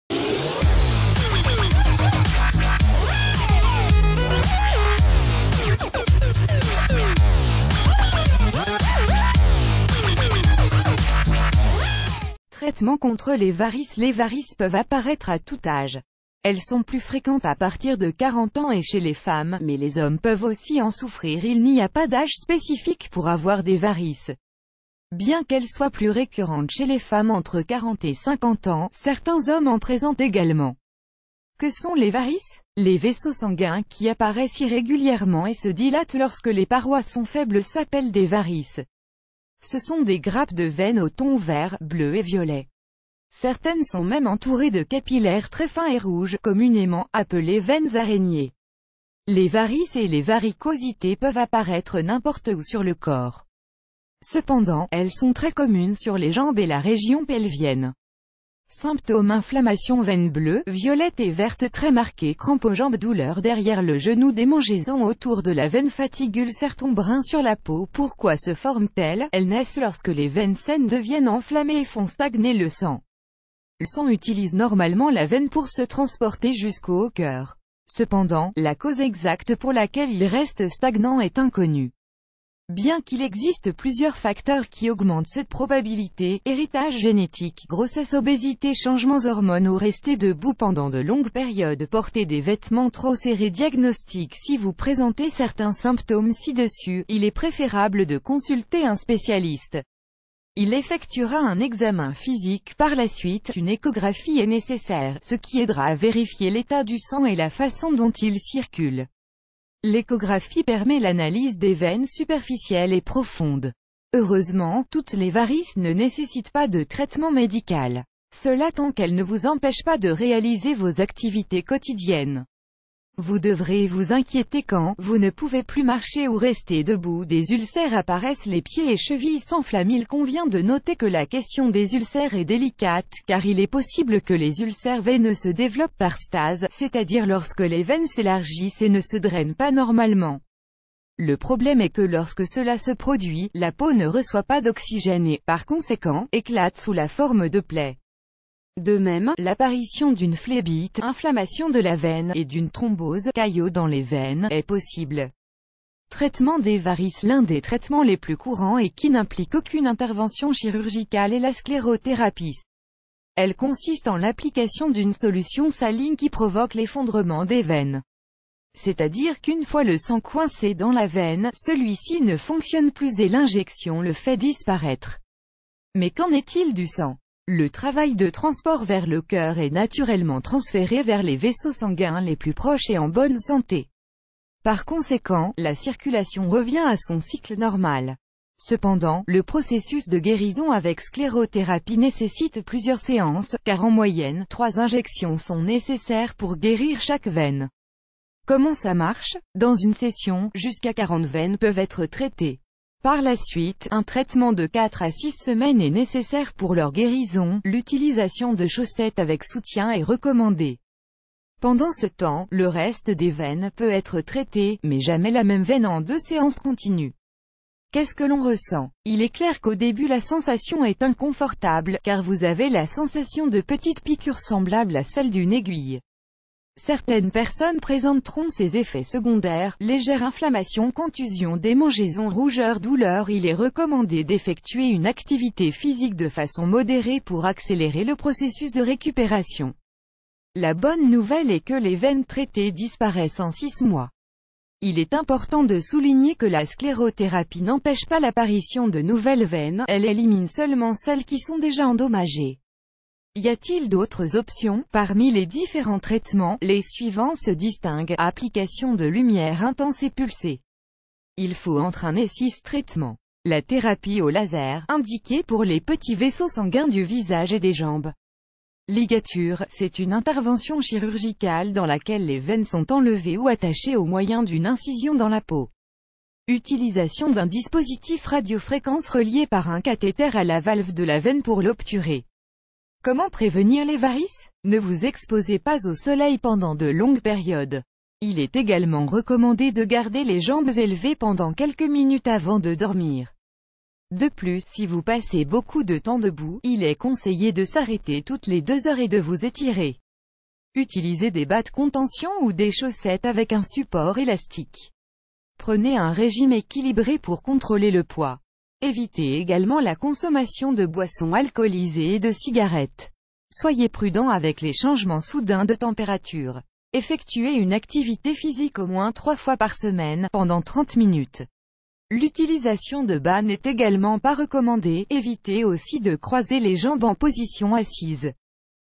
Traitements contre les varices
Traitements contre les varices Les varices peuvent apparaitre à tout âge. Elles sont plus fréquentes à partir de 40 ans et chez les femmes, mais les hommes peuvent aussi en souffrir Il n’y a pas d’âge spécifique pour avoir des varices. Bien qu’elles soient plus récurrentes chez les femmes entre 40 et 50 ans, certains hommes en présentent également. Que sont les varices ? Les vaisseaux sanguins qui...